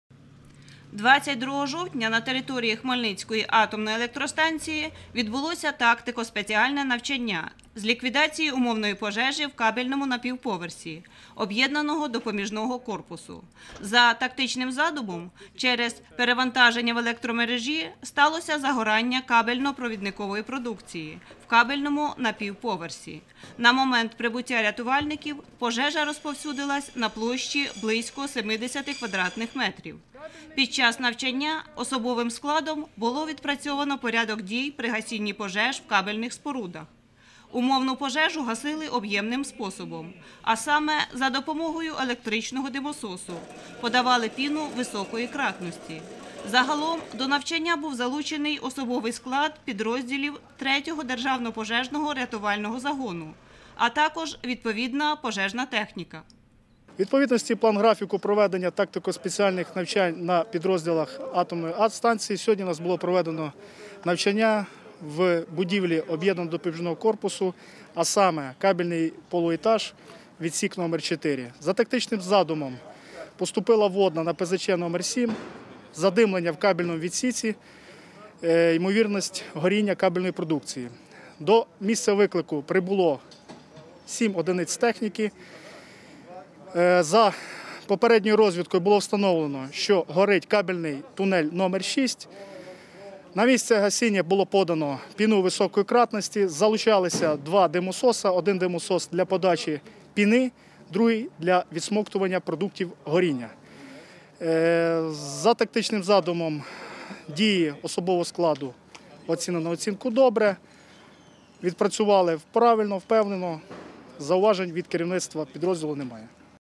23.10.2020.ХАЕС. Відбулися тренування рятувальників
Умовну пожежу успішно подолали